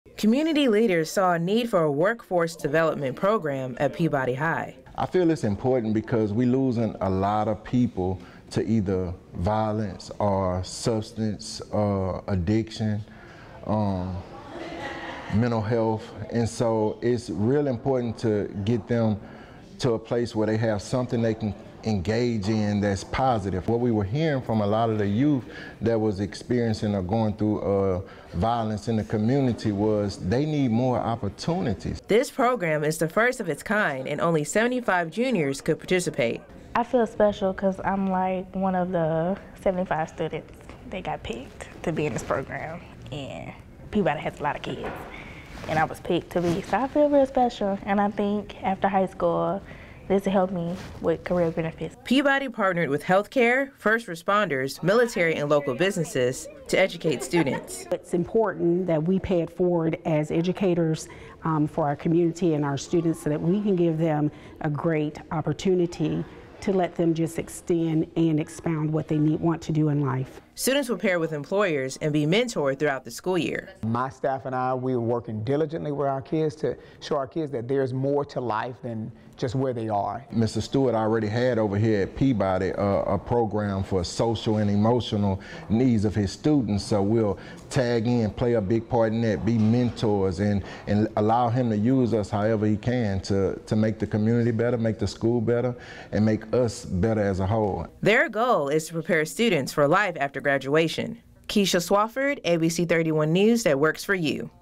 Rapides Parish School Board Initiates Workforce Development Program for Peabody High
The Rapides Parish School Board came up with a solution to close the workforce gap, and their solution is a workforce development program.
ABC 31 News Reporter Keisha Swafford has the story on how this program can prepare students to enter the workforce.